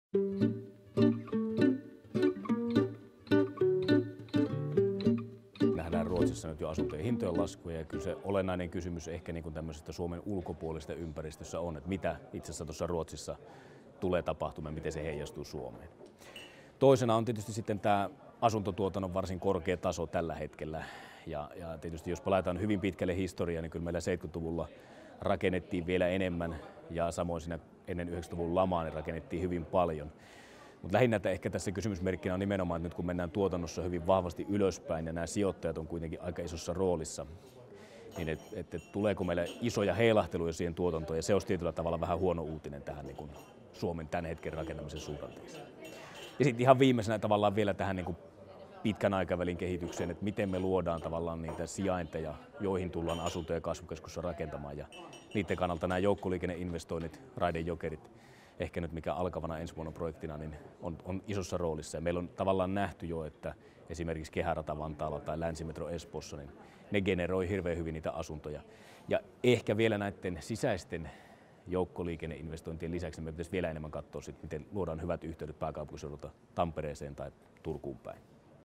Sami Pakarinen: Asuntomarkkinatilanne ja -näkymät
Rakennusteollisuus RT:n pääekonomistin Sami Pakarisen mielestä Suomen asuntomarkkinakehitykseen vaikuttavat erityisesti kolme tekijää: 1) Ruotsin asuntomarkkinatilanteen heijastusvaikutukset 2) Suomen asuntotuotannon sijoittajavetoisuus ja joukkoliikenneratkaisut. Pakarinen esitti arvionsa Asuntomarkkinat 2018 -tapahtumassa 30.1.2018.